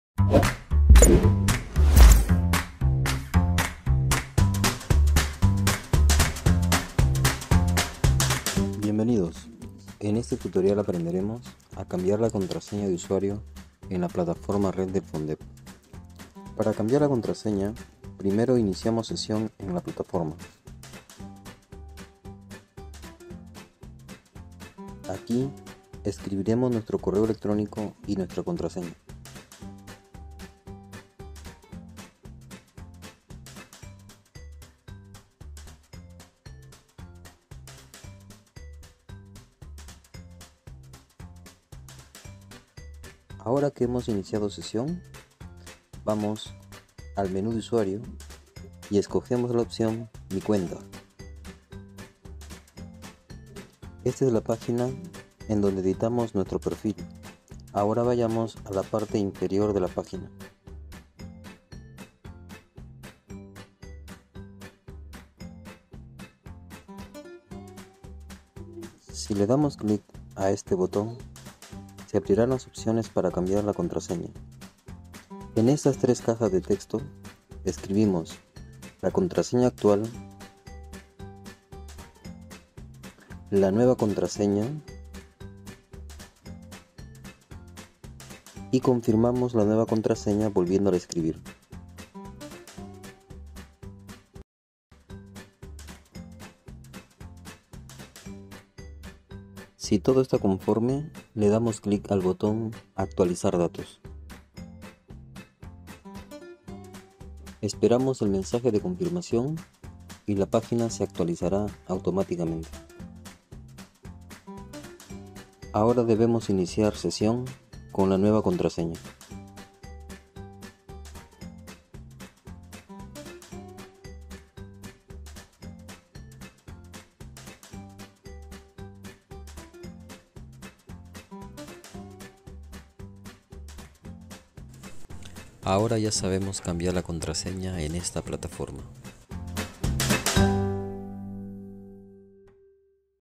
Diplomado “Diseño, gestión y evaluación de proyectos de innovación educativa”: cambio de contraseña
Segunda edición del diplomado de posgrado “Diseño, gestión y evaluación de proyectos de innovación educativa”, organizado por el Fondo Nacional de Desarrollo de la Educación Peruana (FONDEP) en convenio con la Universidad Nacional Micaela Bastidas de Apurímac (UNAMBA), el Gobierno Regional de Apurímac y la Dirección Regional de Educación de Apurímac.